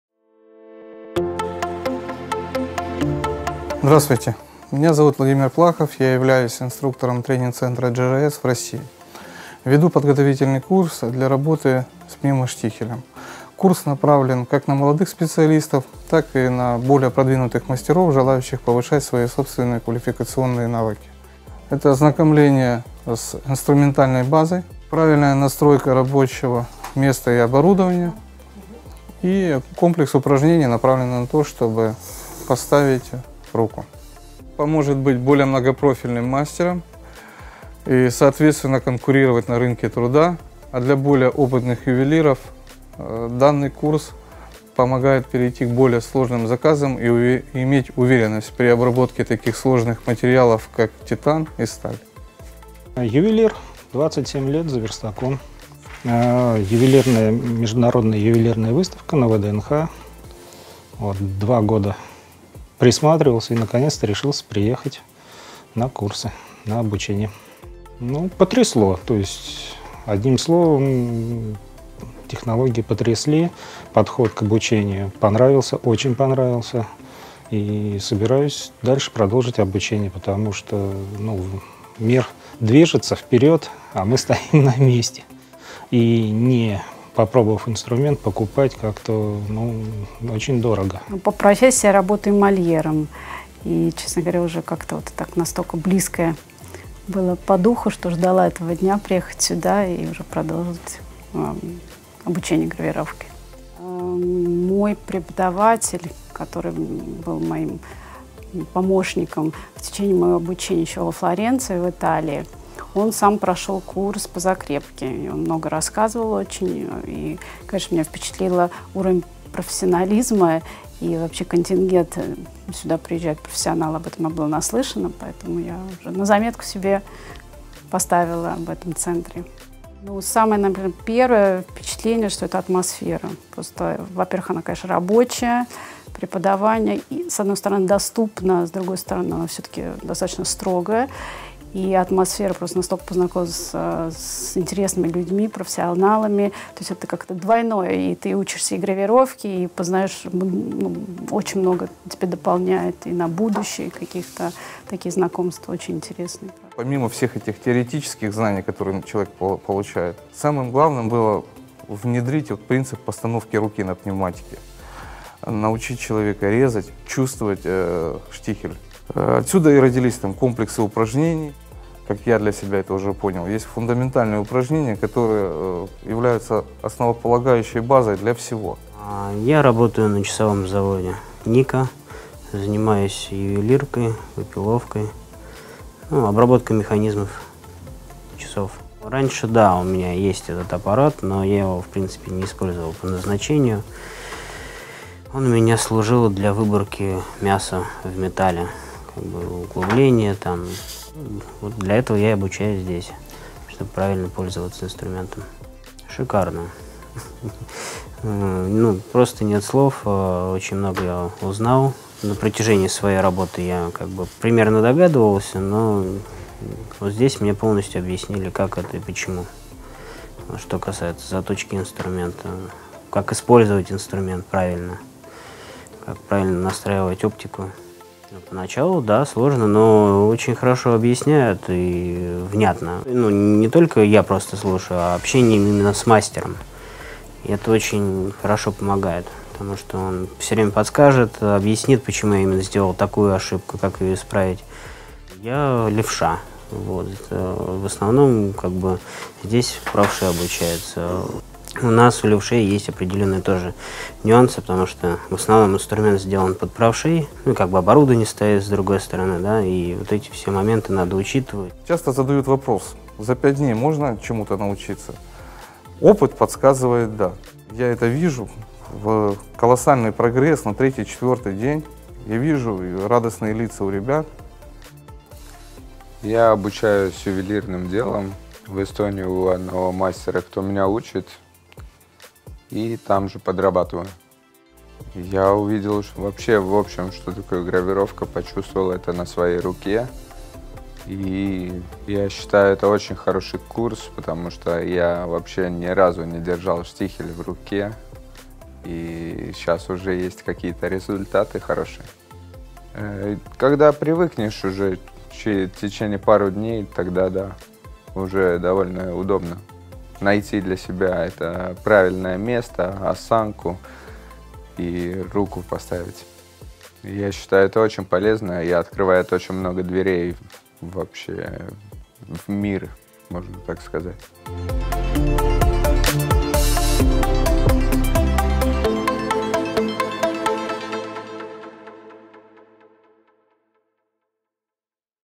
Preparatory Course for Work with GRS Pneumatic Engraving system, GRS Training Center in Russia
This course is aimed to learn basic skills of work with a pneumatic engraving system and also an intensive training for hands' setup.